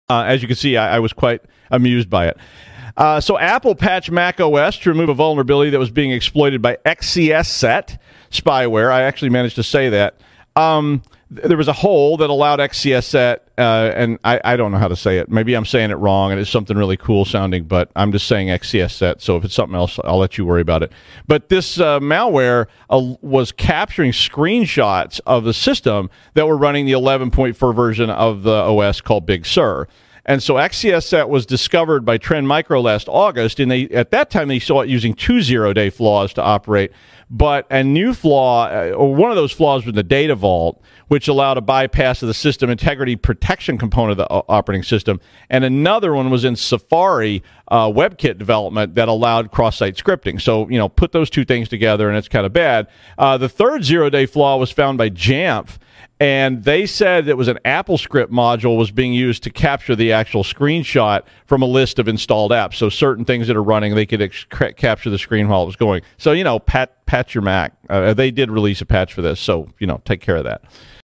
Apple 0-Days Allow For Spying!
Several Apple 0-Days were disclosed that allow attackers to take remote screenshots and grab video footage from the webcam.